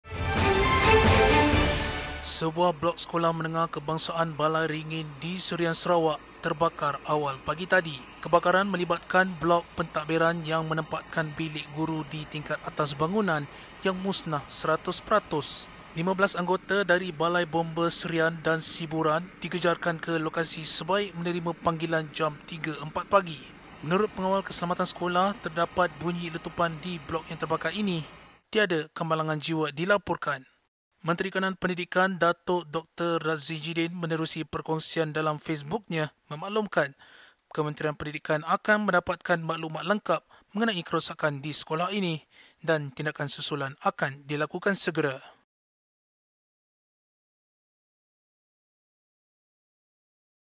Bilik Guru Musnah
Sebuah blok Sekolah Menengah Kebangsaan Balai Ringin di Serian, Sarawak terbakar awal pagi tadi.

Kebakaran melibatkan blok pentadbiran yang menempatkan bilik guru di tingkat atas bangunan yang musnah 100 peratus.